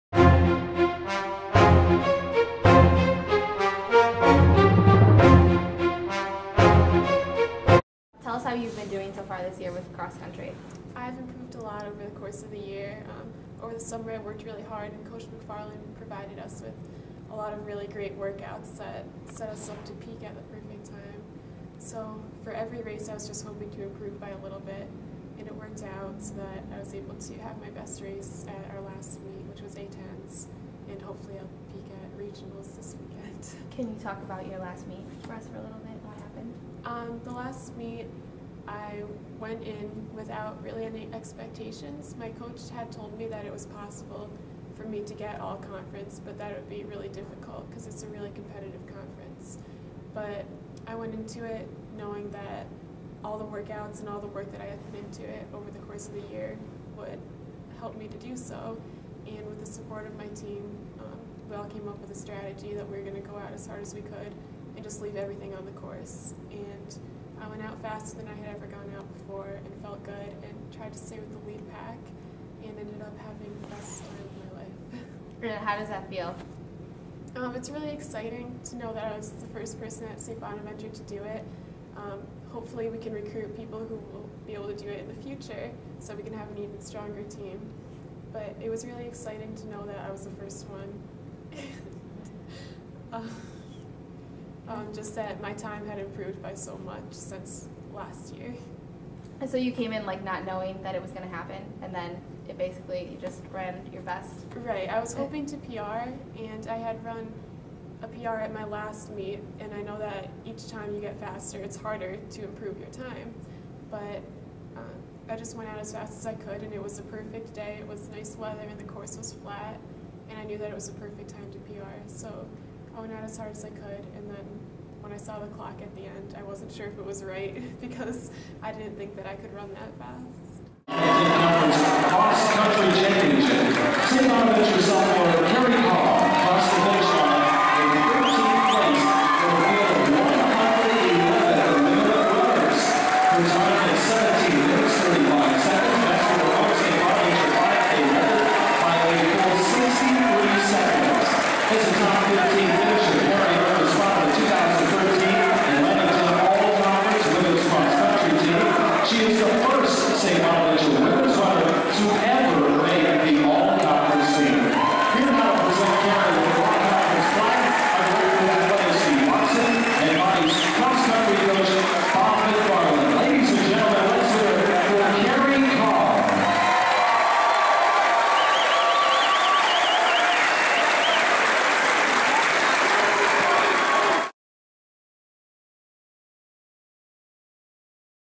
Kerry Caher Interview
Feature Video on Cross Country runner Kerry Caher who finished in 11th place at the 2013 Atlantic 10 Championship becoming the first SBU women's runner to earn All-Conference.